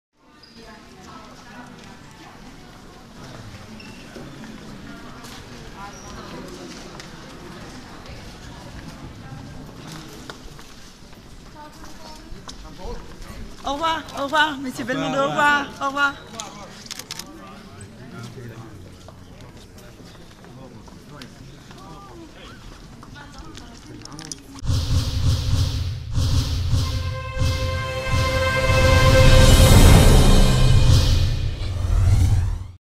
BELMONDO U KOLICIMA NA CRVENI TEPIH: Slavni glumac stigao nasmejan i oduševio fanove
Vidimo se i na Viberu ili Vajberu, kako se kome više sviđa